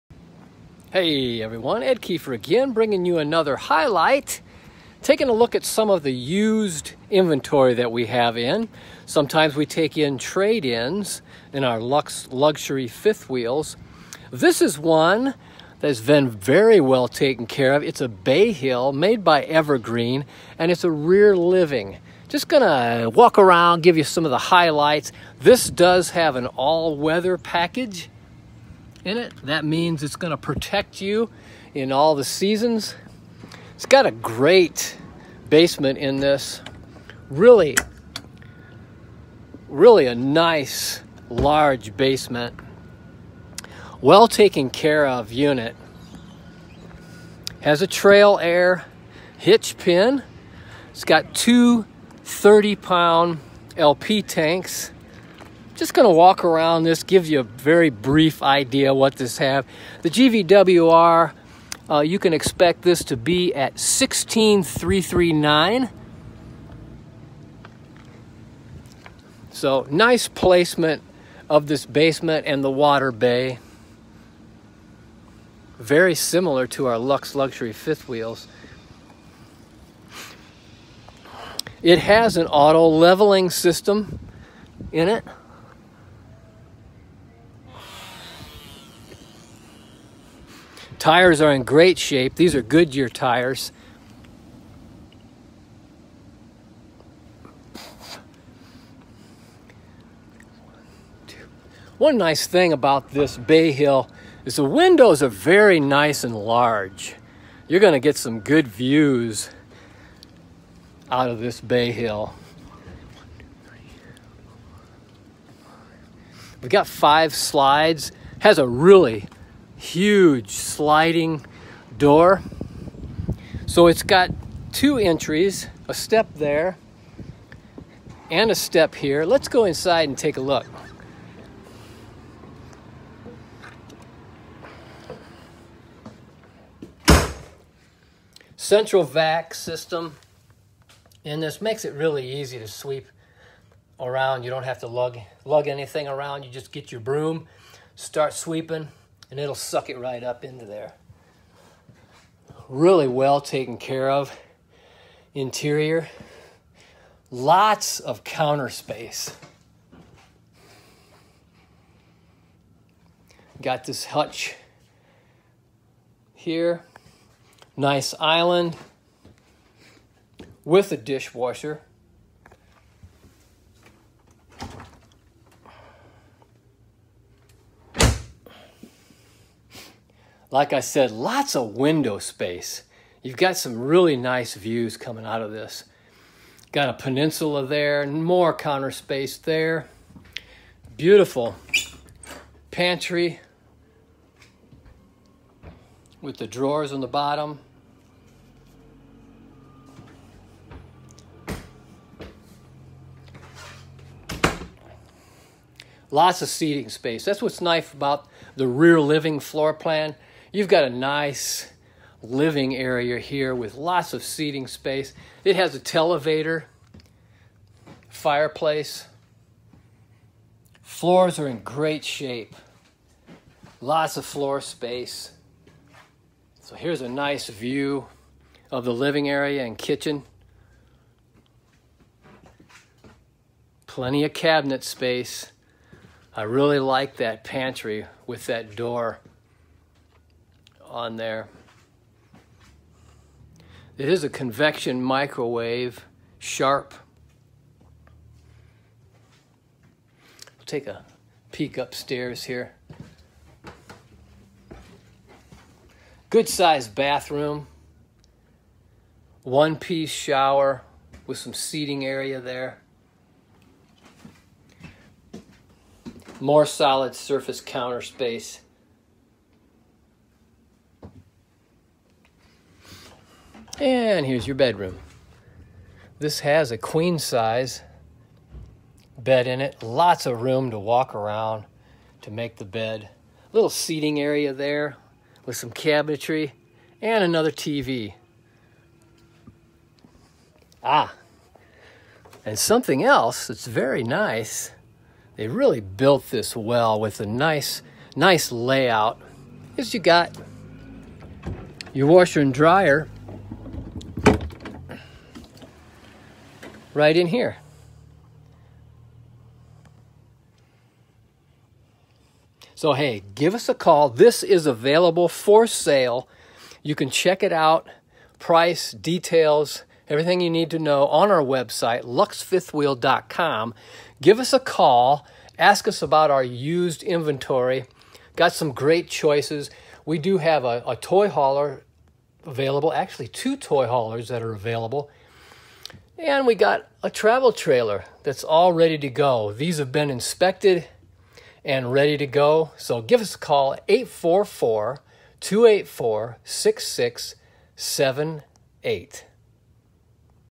2016 Evergreen Bayhill 375RE - 1079
2016 Evergreen Bayhill 375RE - 1079

Total Dry Weight 13,350
Dry Hitch Weight 2,500
GVWR (LBS) 16,500
Fresh Water Capacity (GAL) 70
Gray Water Tank Capacity (GAL) 70
Black Water Tank Capacity (GAL) 40
LP Capacity (LBS) 60
Cargo Carrying Capacity (LBS) 2,400
Overall Length 41’ 4”
Overall Height (including AC) 13’-1”
Width 100”
Bed Size (inches) 60 x 80
Power Awning Length 16'
Tire Size ST235/80R16
Awning
Slide toppers
Leveling Jacks
Year: 2016